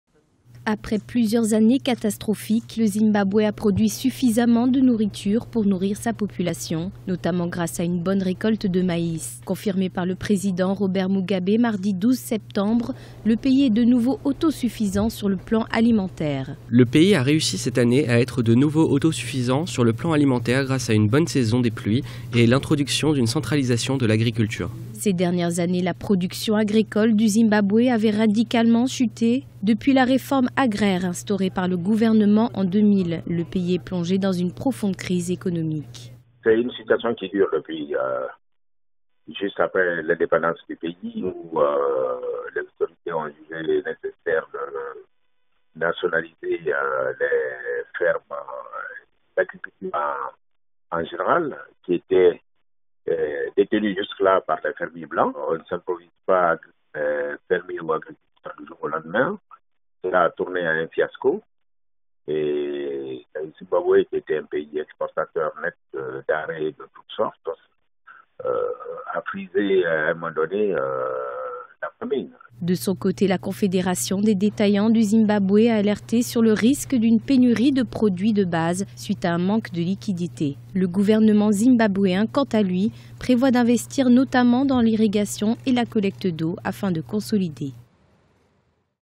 Zimbabwe, LA RÉFORME AGRAIRE PORTE SES FRUITS
Politique, Proclamation de l'autosuffisance alimentaire